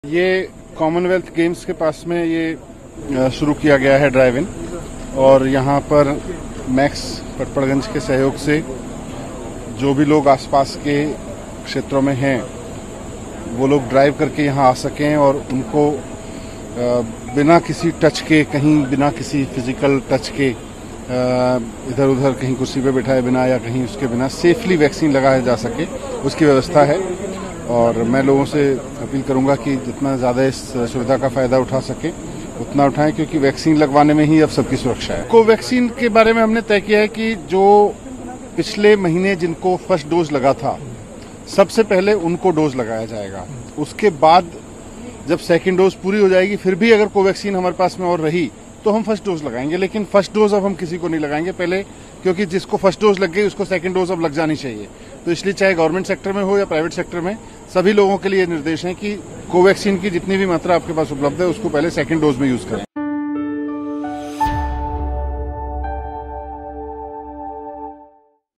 Coronavirus Vaccine Shortage: Delhi में अब First Dose में नहीं लगेगी Covaxin। Sisodia। कोरोनावायरस
दिल्ली  के कॉमनवेल्थ गेम्स विलेज में सोमवार से ड्राइव थ्रू वैक्सीनेशन कैंप की शुरुआत हुई। इसका उद्घाटन दिल्ली के उपमुख्यमंत्री मनीष सिसोदिया ने किया। इस दौरान कई लोगों का कार में बैठे-बैठे कोरोना वैक्सीन लगाई गई जिसका सिसोदिया ने जायजा भी लिया। मीडिया से बातचीत में सिसोदिया ने बताया कि  कोवैक्सीन की कमी को देखते हुए अब ये तय किया गया है कि उसे सिर्फ दूसरे डोज में ही लगाया जाएगा। सभी अस्पतालों और वैक्सीन सेंटर्स को ये निर्देश दे दिया गया है कि कोवैक्सीन पहली डोज में ना लगाई जाए। अगर दूसरी डोज लगाने के बाद कोवैक्सीन बचती है, फिर उसे पहली डोज के तौर पर देने को लेकर विचार किया जाएगा।